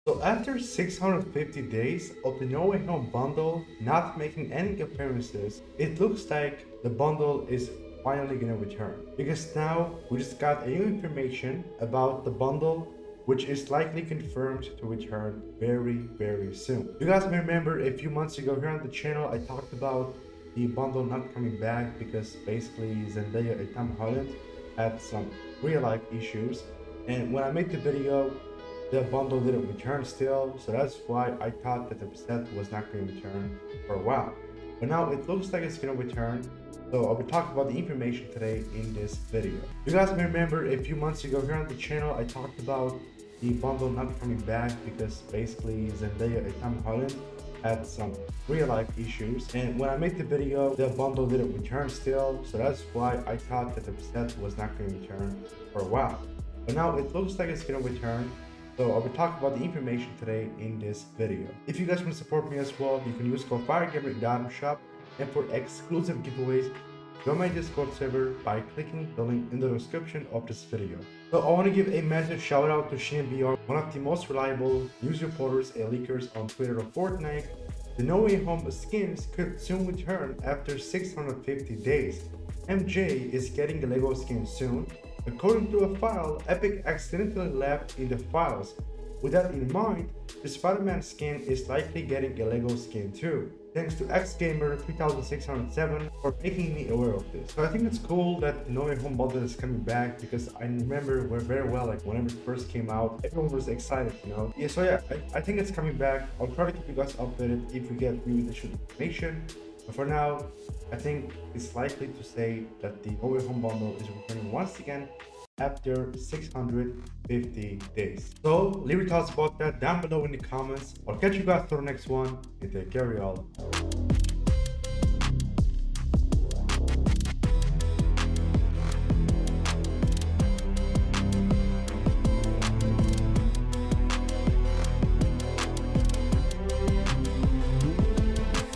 Fortnite x No Way Home Is Returning Soon!
MJ, part of the No Way Home bundle, is receiving its own lego style soon, along with spider-man.

🌟This Video Is Made & Owned By Firegamer V2!